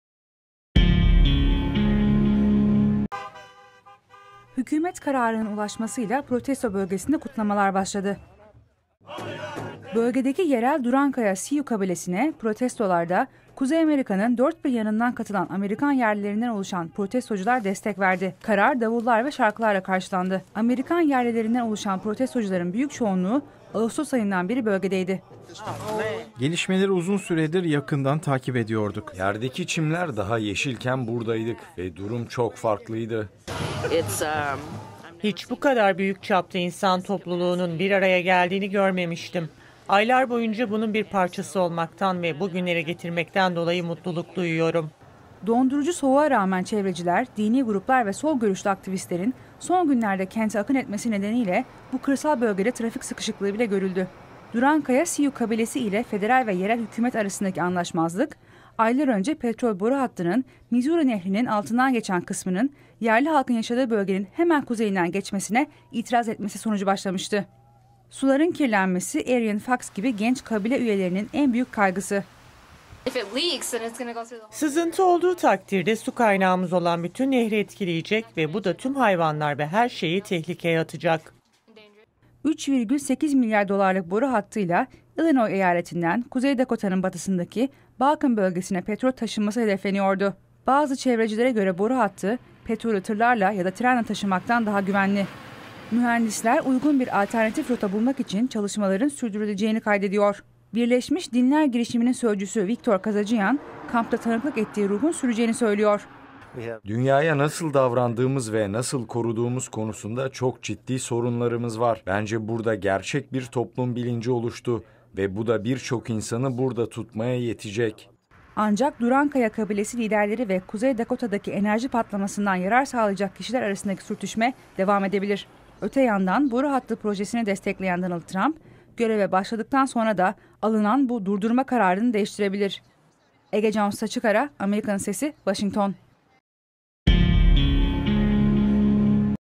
Amerikan Yerlilerinin Kuzey Dakota Zaferi
Kuzey Dakota eyaletinde inşası devam eden petrol boru hattının tartışmalı 2 kilometrelik bölümünün federal hükümet tarafından durdurulması, aylardır zor koşullarda inşaatı protesto binlerce kişi tarafından sevinçle karşılandı